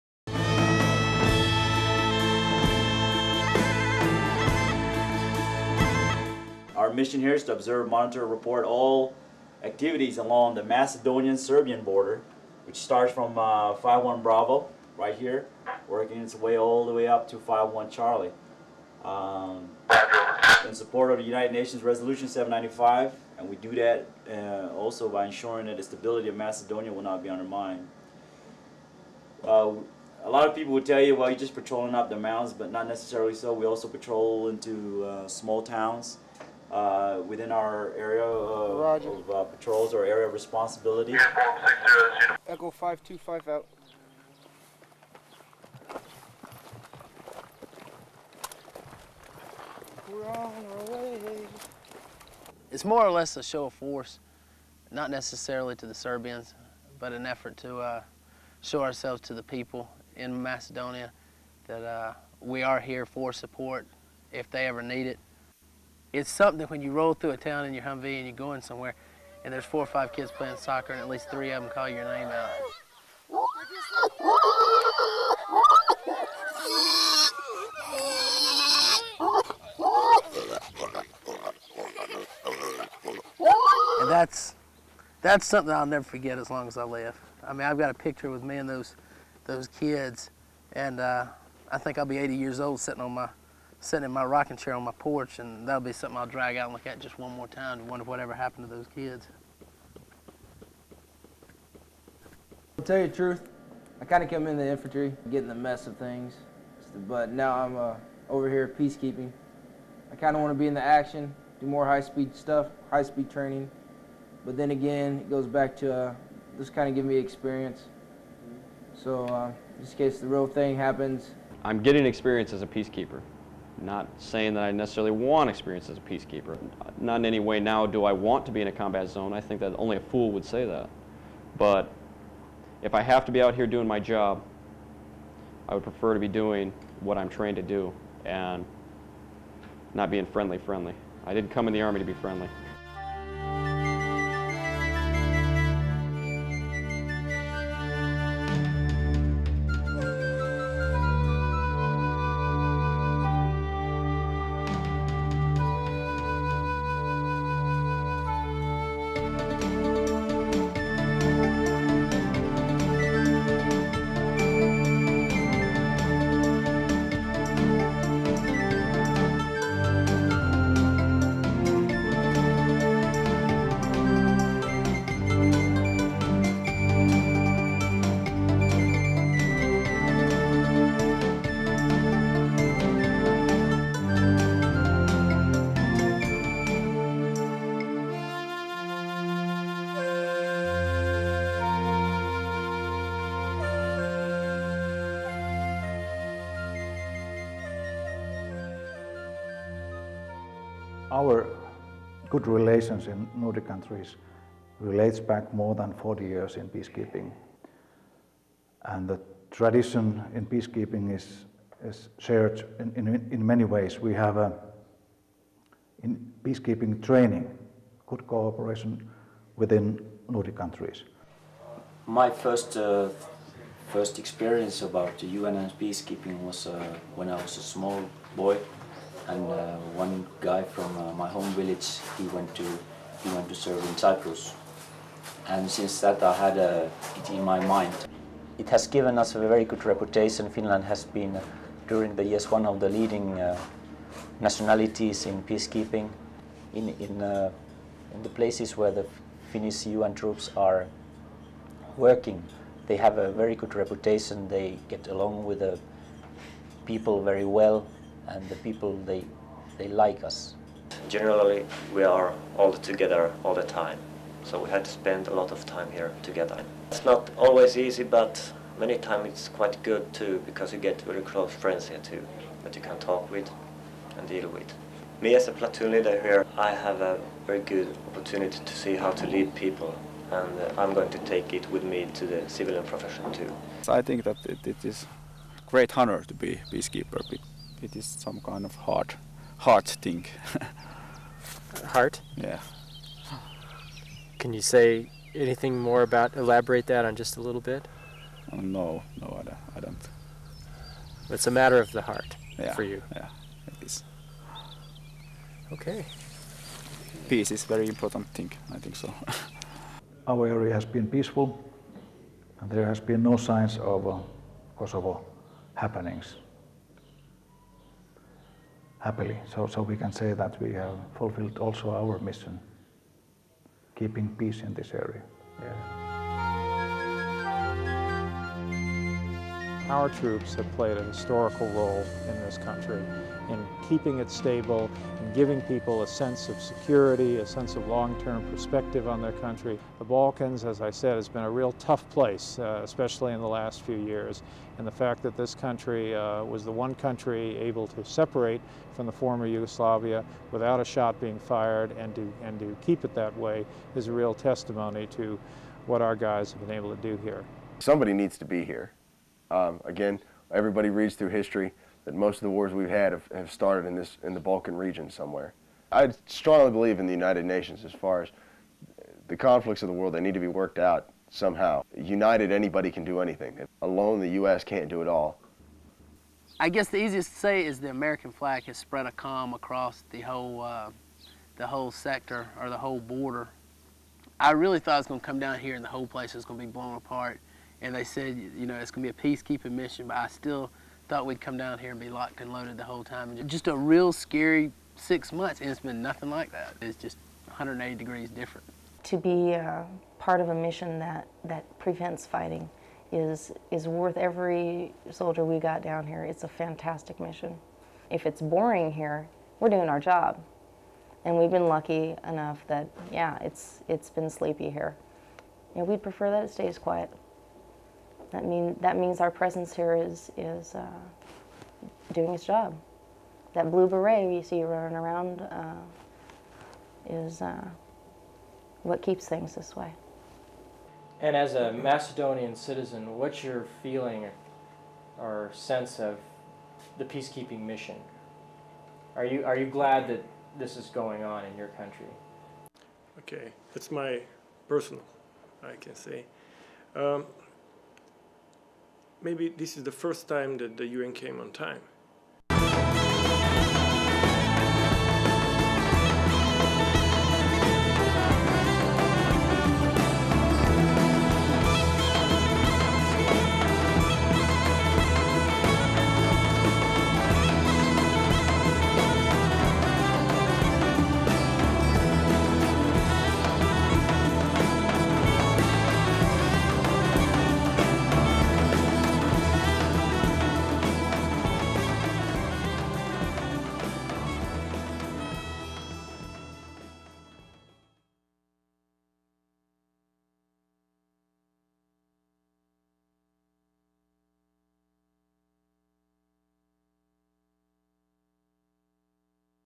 "On Tour with the Blue Berets" (Part 2)
Our deployment in Macedonia was successful. In spite of the Kosovo War that followed in 1999, there has been no war on Macedonian soil. This coming spring my partner in the production, Stephen Maly, and I hope to return there to do a follow-up piece, interviewing many of the same people.
These two segments are just part of our final one-hour documentary. It's good to remember how we once promoted peace and succeeded - without firing a single shot.